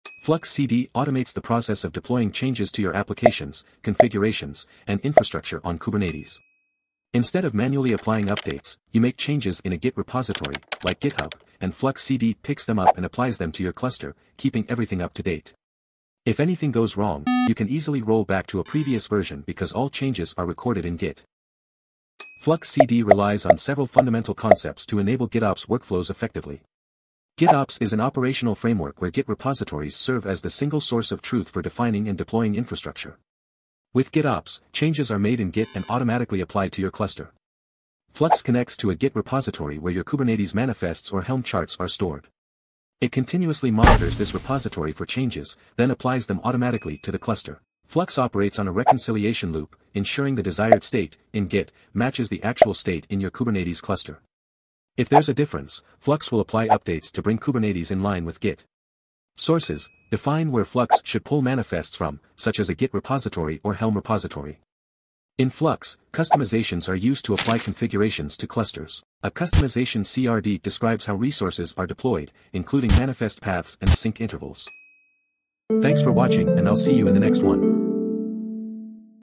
FluxCD in 100 Seconds
This video tutorial explains what is Flux CD and its core concepts like GitOps, Git Repository, Reconciliation Loop, Sources and Custom Resources (CRDs) and Kustomizations.